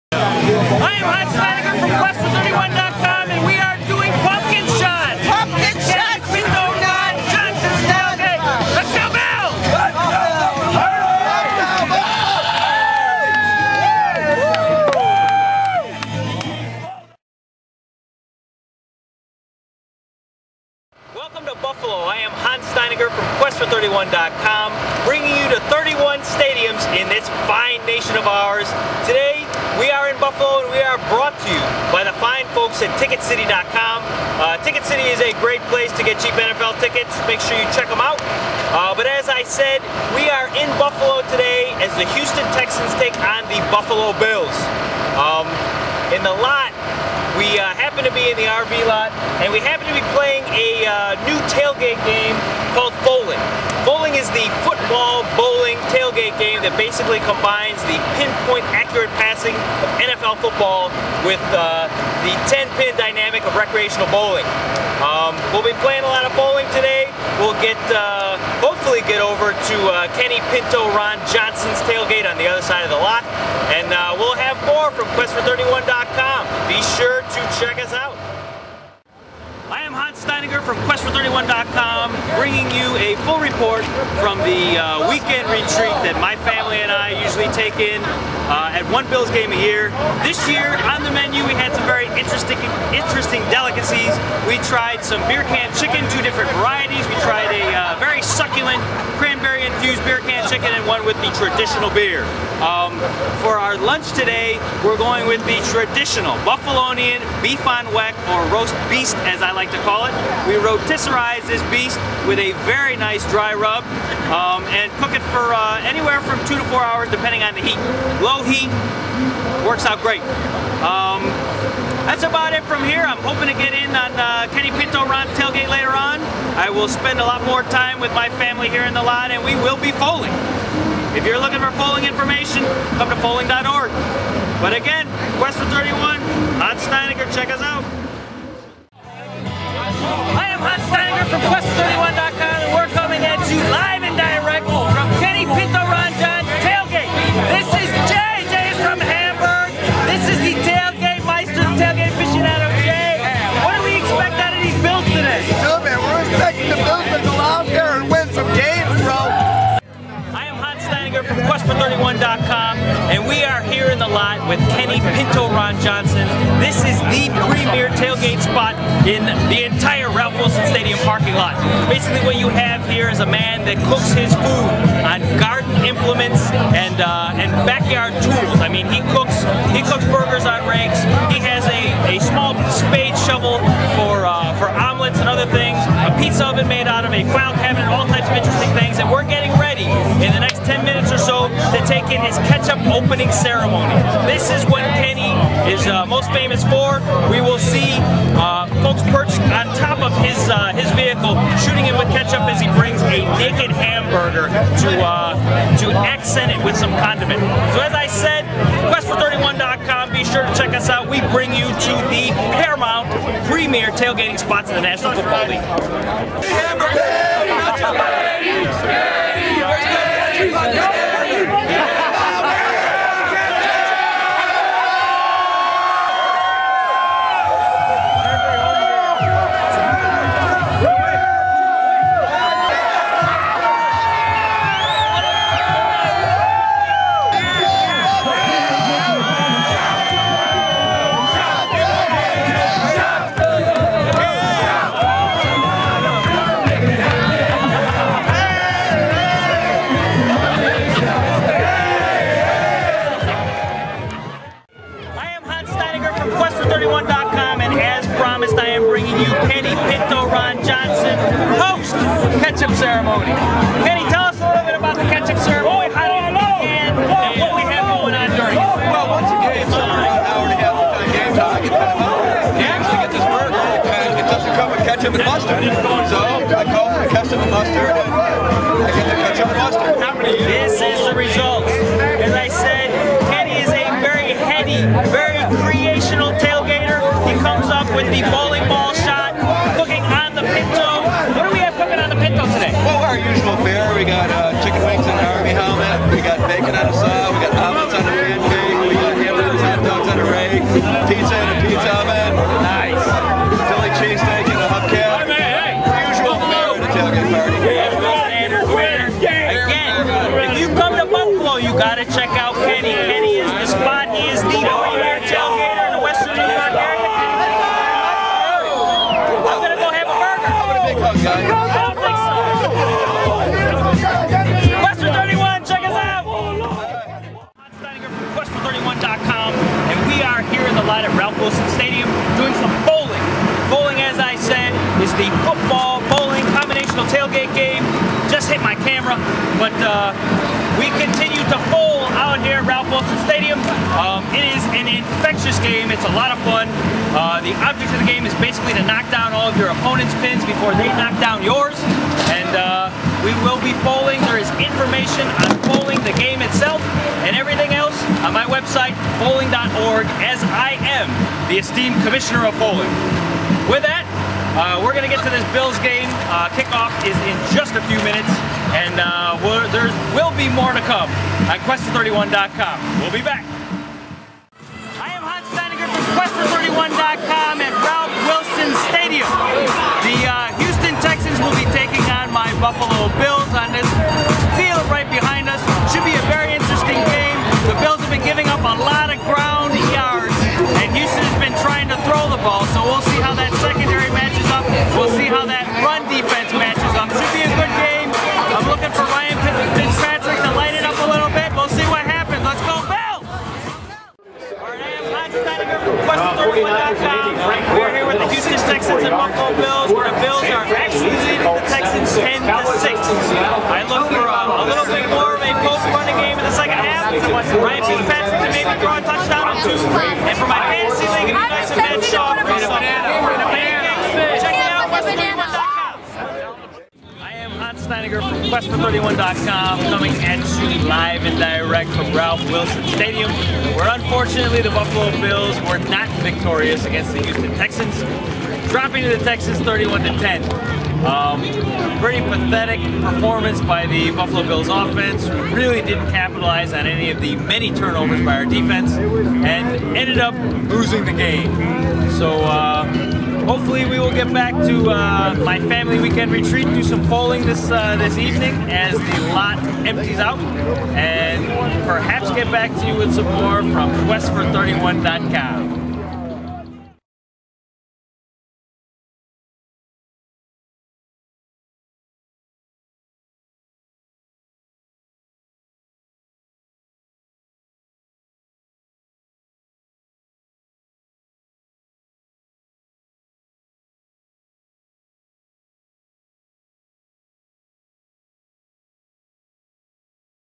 Questfor31-Buffalo-Nov2009.wmv
Buffalo, New York and Ralph Wilson Stadium are the site of yet another Quest for 31 stadium review.  Watch Hans Steiniger as he brings the football-bowling tailgate game of Fowling to western New York.  Hang out with Kenny Pinto Ron Johnson as he gets sprayed with Ketchup and check in with the Annual Western New York Family Weekend Retreat.